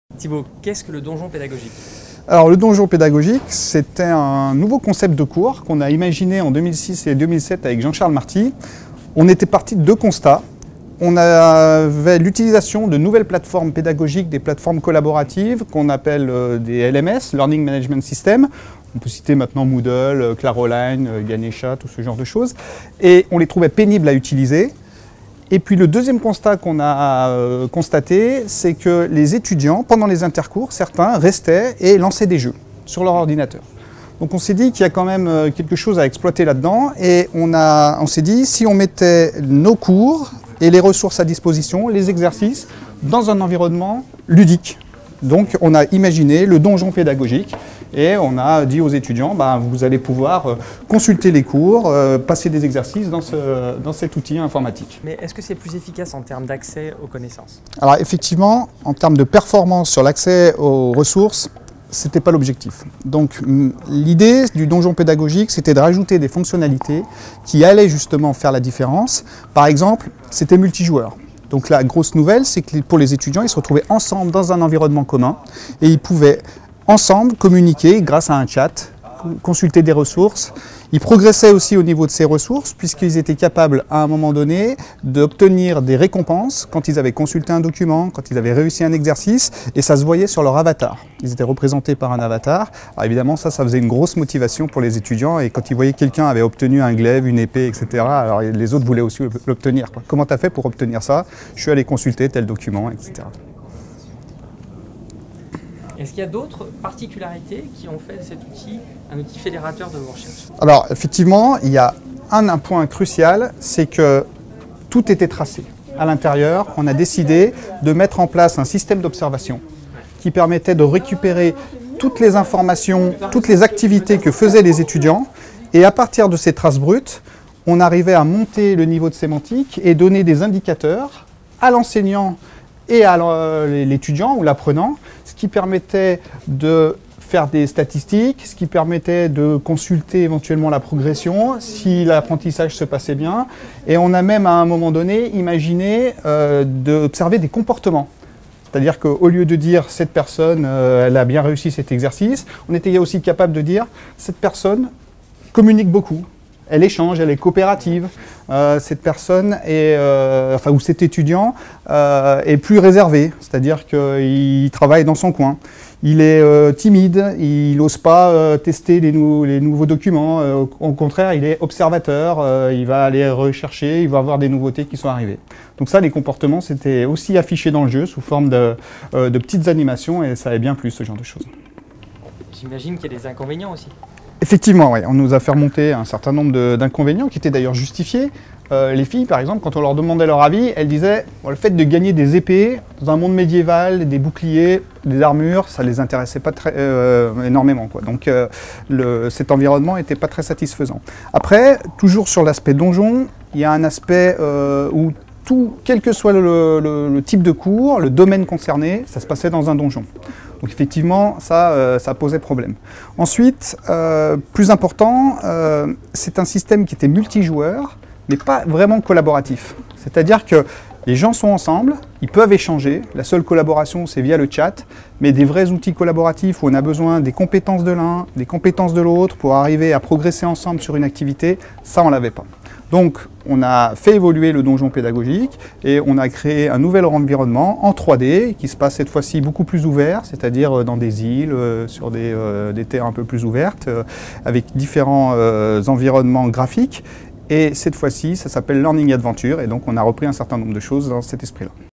Donjon Pédagogique
Interview de THIBAULT CARRON au sujet du Donjon Pédagogique